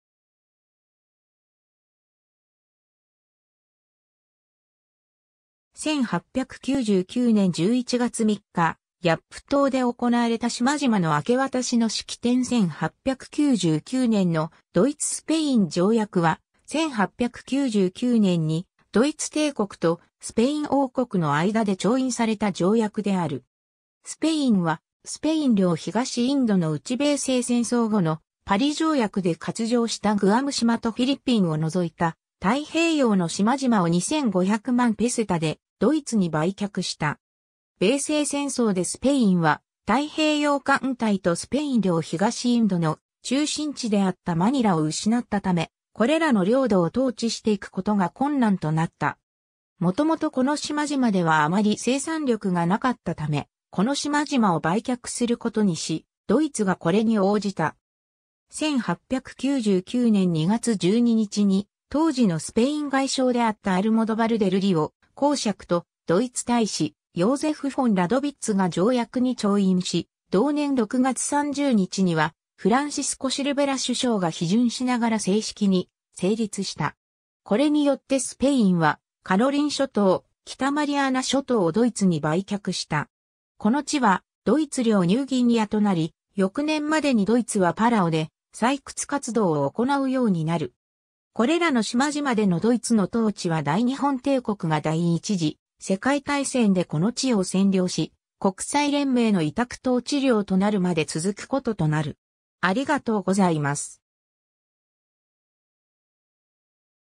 ドイツ・スペイン条約 (1899年)
1899年11月3日、ヤップ島で行われた島々の明け渡しの式典 1899年のドイツ・スペイン条約(スペイン語: Tratado germano-español de 1899; ドイツ語: Deutsch-Spanischer Vertrag 1899)は、1899年にドイツ帝国とスペイン王国の間で調印された条約である。
スペインはスペイン領東インドのうち米西戦争後のパリ条約で割譲したグアム島とフィリピンを除いた太平洋の島々を2500万ペセタでドイツに売却した。
米西戦争でスペインは太平洋艦隊とスペイン領東インドの中心地であったマニラを失ったため、これらの領土を統治していくことが困難となった。
元々この島々ではあまり生産力がなかったためこの島々を売却することにし、ドイツがこれに応じた。
1899年2月12日に当時のスペイン外相であったアルモドバル・デル・リオ公爵とドイツ大使ヨーゼフ・フォン・ラドヴィッツが条約に調印し、同年6月30日にはフランシスコ・シルヴェラ首相が批准しながら正式に成立した。
 これによってスペインはカロリン諸島、北マリアナ諸島をドイツに売却した。
この地はドイツ領ニューギニアとなり、翌年までにドイツはパラオで採掘活動を行うようになる。
 これらの島々でのドイツの統治は大日本帝国が第一次世界大戦でこの地を占領し、国際連盟の委託統治領となるまで続くこととなる。